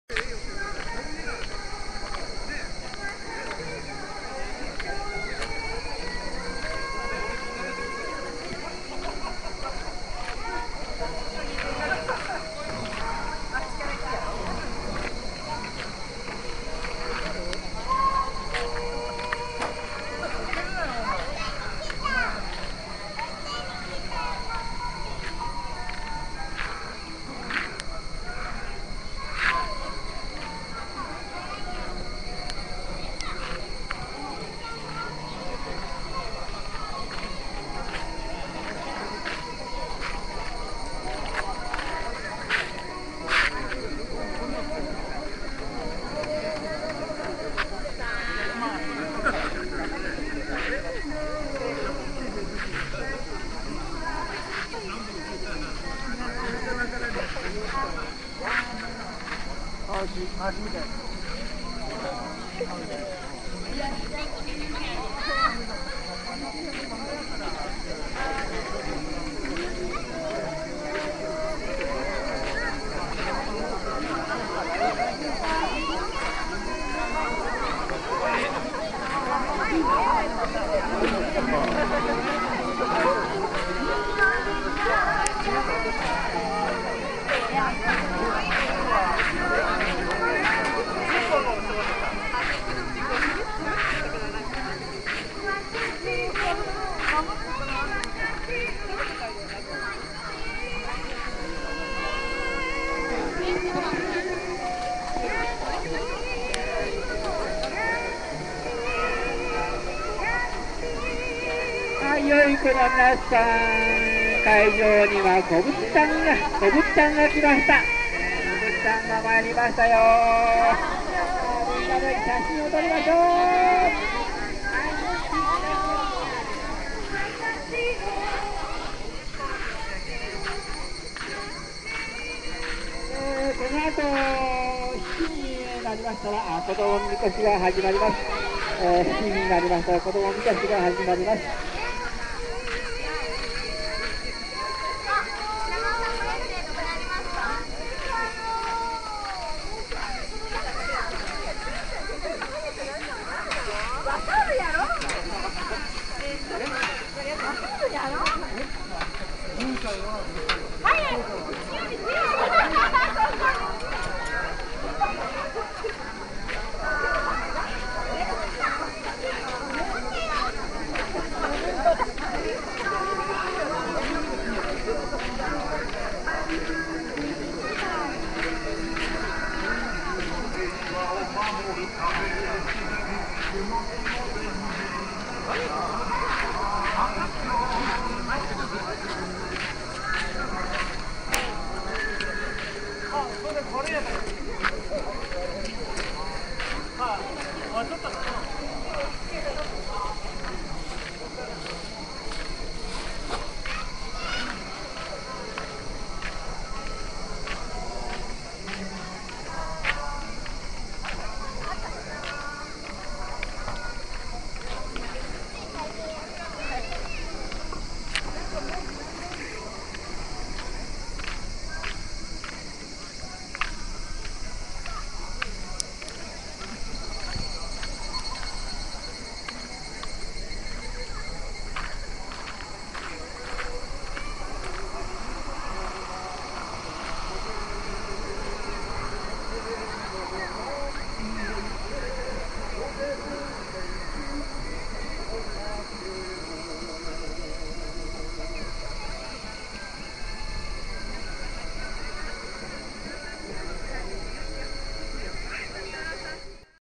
H２６年 美祢市 岩永地区ふるさと夏祭り大会
８月１３日に開催された岩永地区ふるさと夏祭り大会です。